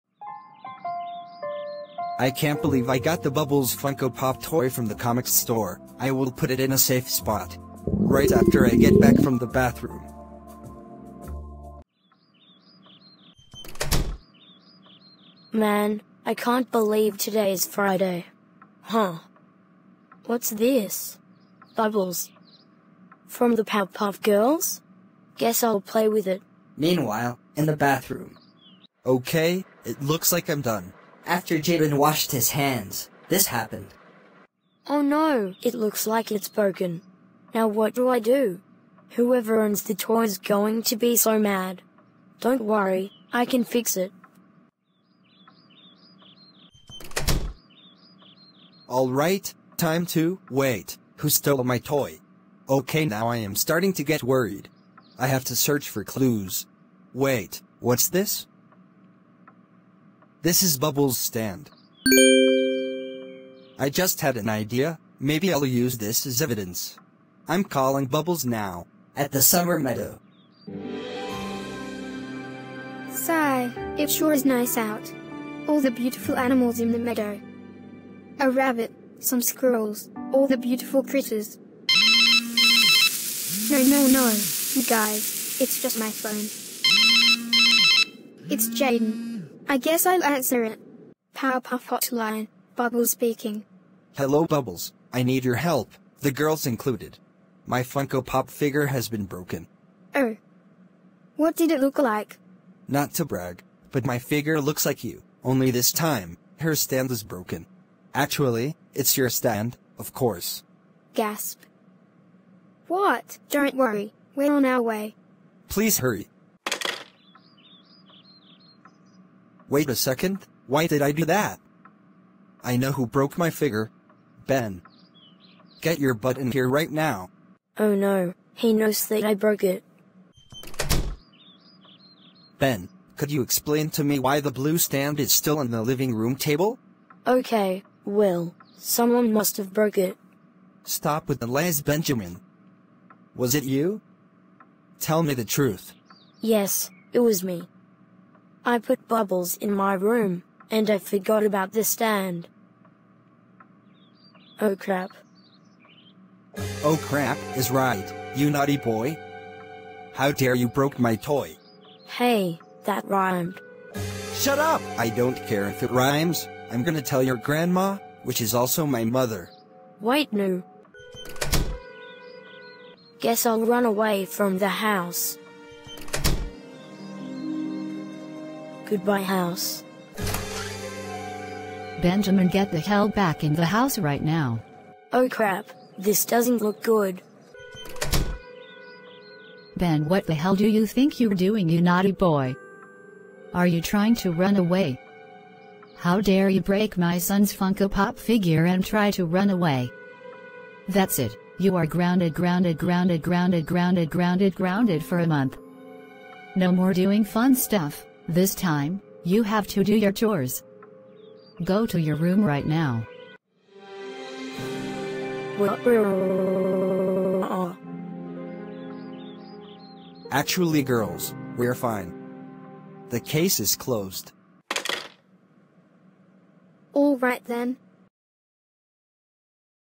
Ben Broke Jaydon’s PPG Funko Pop Figure/Tries To Run Away the House/Gets Grounded
Directed by YOU!

For @ninjaproyt4095 I never realized how you broke my toy, so here, I made this video for you!

In other words, hope you enjoy the video.